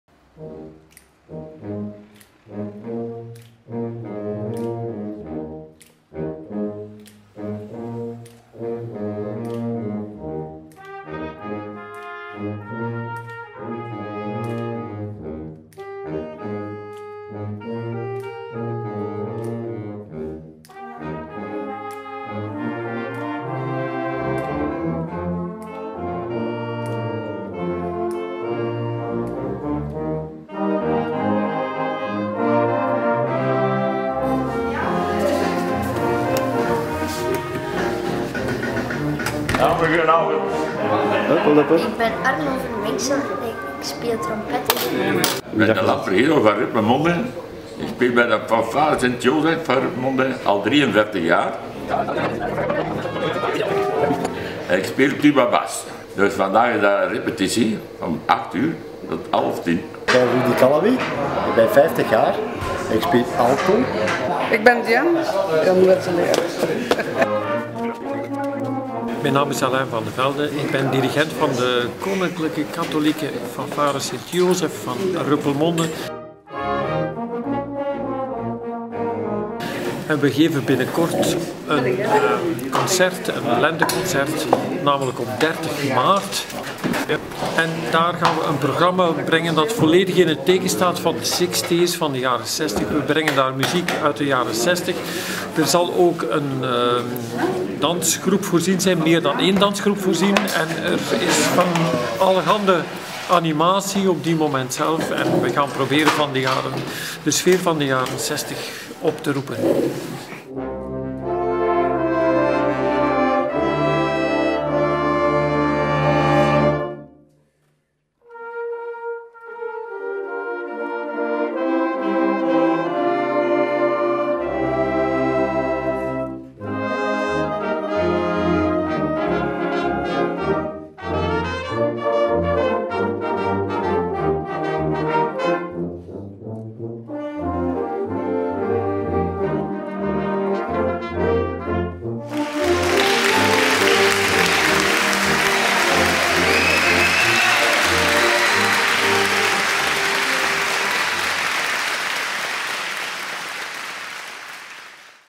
50ste lenteconcert KK Fanfare Sint-Jozef Rupelmonde
Promotiefilmpje voor het concert van de Koninklijke Katholieke Fanfare Sint-Jozef uit Rupelmonde voor hun 50ste lenteconcert op 30 maart 2014 in de feestzaal van 't Gildenhuis te Rupelmonde. Aanvang 15u00. Voorverkoop bij Studio Deyaert en in 't Gildenhuis.